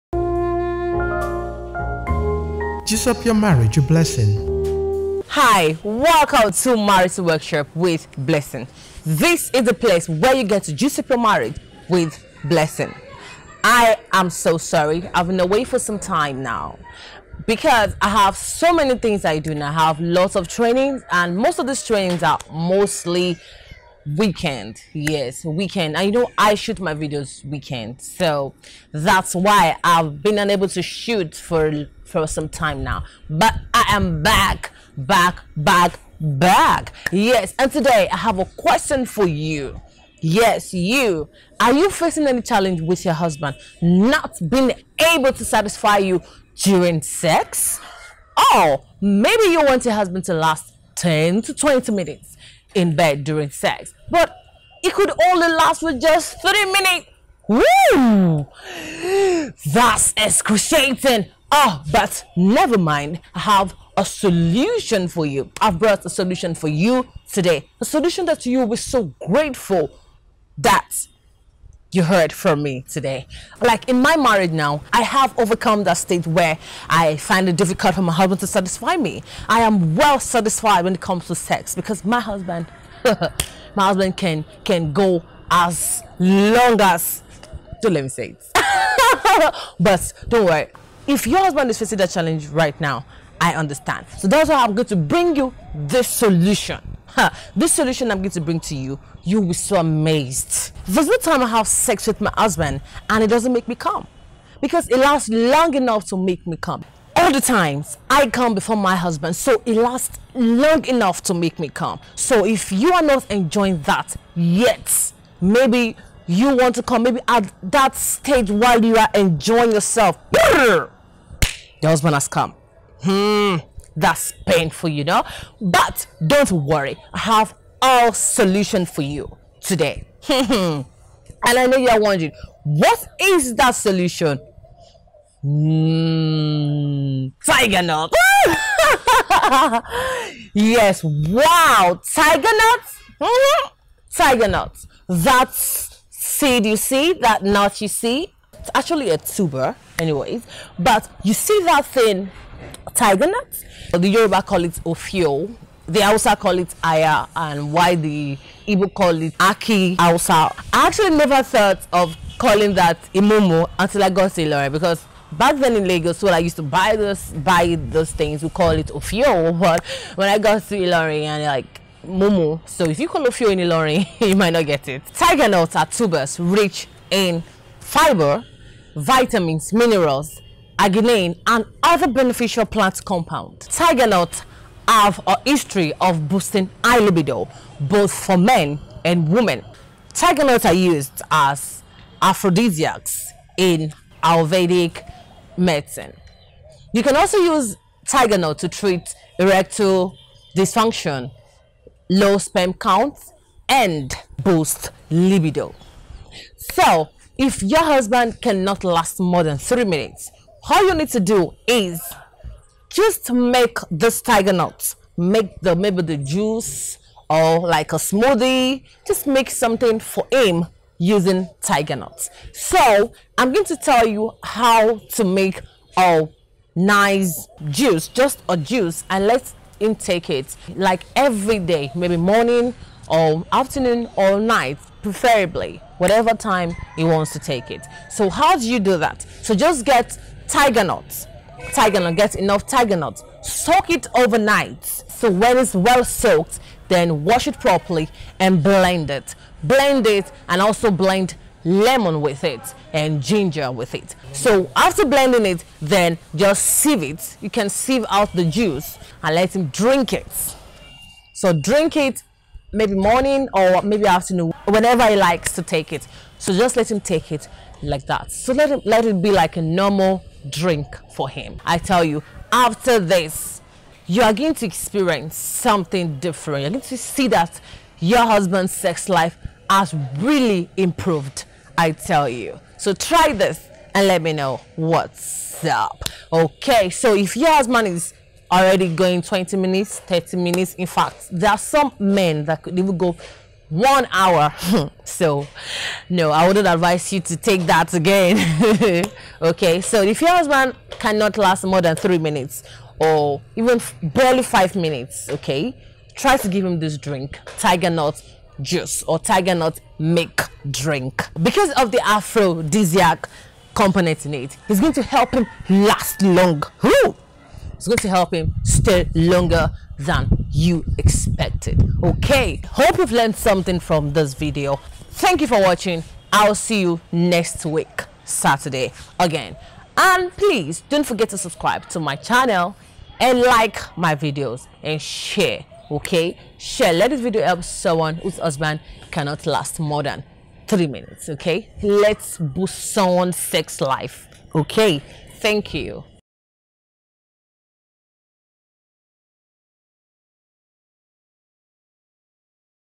DIY Tiger Nut Drink to last long during sex till she Orgasms by Jaaziah Blessing on Marital Workshop
DIY Tiger Nuts Drink to last long in bed during sex till she Orgasms by Jaaziah Blessing on Marital Workshop
Tiger nuts is rich in fibre, has 20-30 per cent tiger-nut oil which is nourishing to the epidermis, nullifies hard-knots in the stomach and acts as a coolant to hot flushes associated with premature menopause.

Sexual performance
Over the years, people have used tiger nuts to treat erectile dysfunction (ED). Although there is no scientific proof, some men drink tiger nut milk before sexual intercourse. this video by Jaaziah blessing on marital workshop will show you how to make your own tiger nuts drink at home so that you can start satisfying your wife long enough in bed during sex